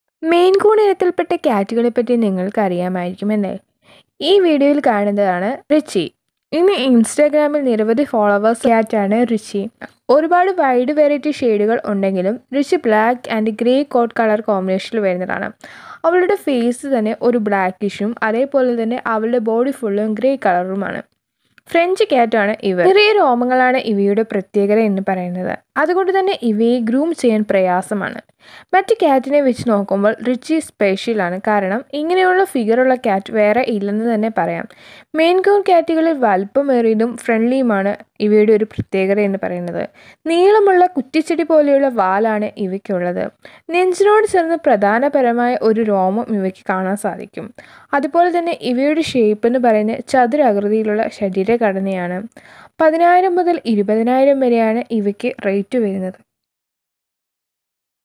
ഈ പൂച്ചകൾ മിടുക്കൻമാർ തന്നെ ||Maine Coon🐈🐈
Sometimes weighing up to 25 pounds, the Maine Coon is considered one of the largest domestic cat breeds. Nevertheless, they are good natured and gentle. They thrive in families with children and other pets, even dogs. Additionally, they are known to be very tolerant and can easily adapt to the needs of children.